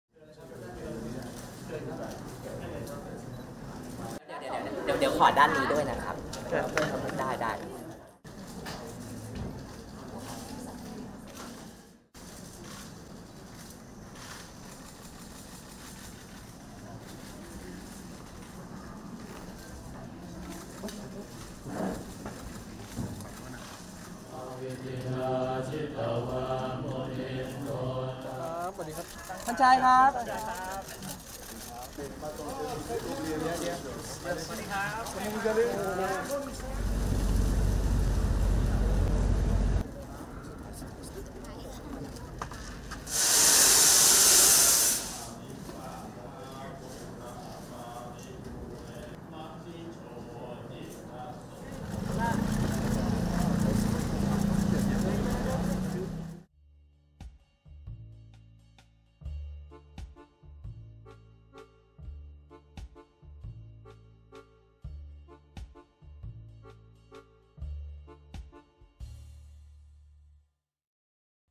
タイ国王の息子、寺院訪問 突然米国から帰国で注目（2023年8月撮影）
タイのワチラロンコン国王の次男ワチャレーソーン氏（42）が8月10日、バンコク市内の寺院を訪れた。ワチャレーソーン氏は20年以上滞在した米国から帰国したばかりで、注目が集まっている。

ワチャレーソーン氏の母親は国王の2番目の妻だった元俳優スチャーリニー・ウィワチャラウォンさん。1990年代に両親が離婚した後は、人生の大半を国外で過ごしてきた。現在は米ニューヨークで弁護士として働いている。